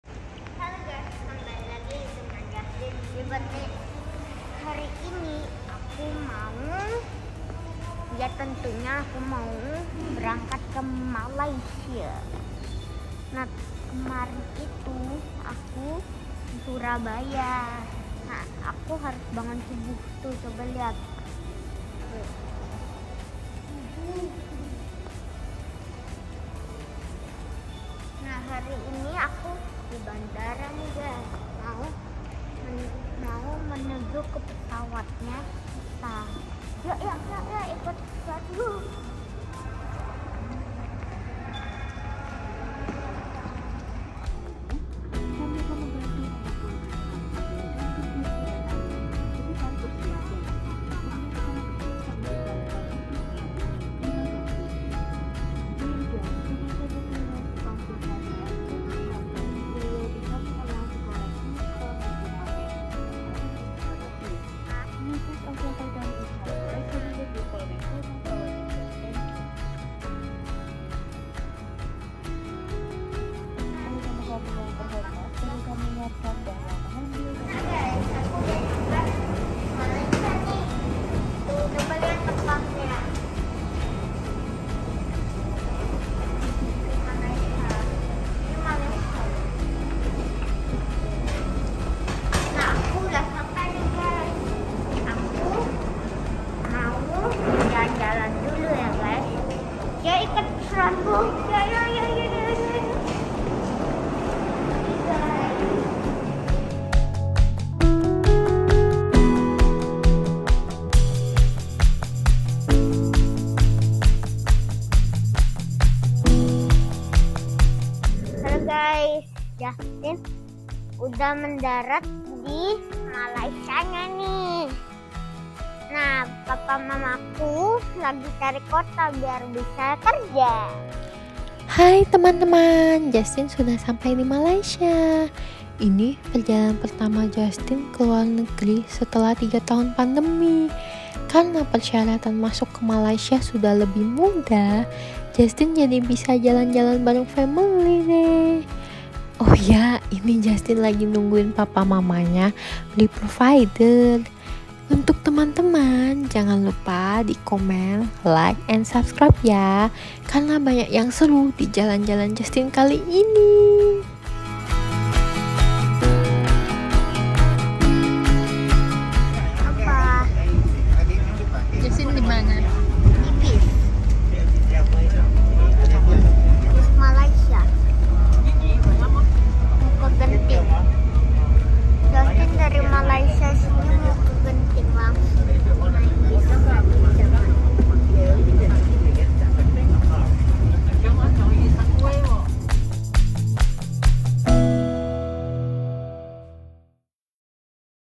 Goes To Malaysia | Keluar Negeri Saat Pandemi | Vlog #5
Hai Guysss…

Setelah 3 tahun, pandemi Covid akhirnya mereda..
Justin senang sekali bisa jalan2 lagi ke luar negeri bersama keluarga, tentu saja dengan tetap mengikuti protokol kesehatan yaa..
Semoga kita semua selalu sehat dan bahagia😁

Xoxo,
Justin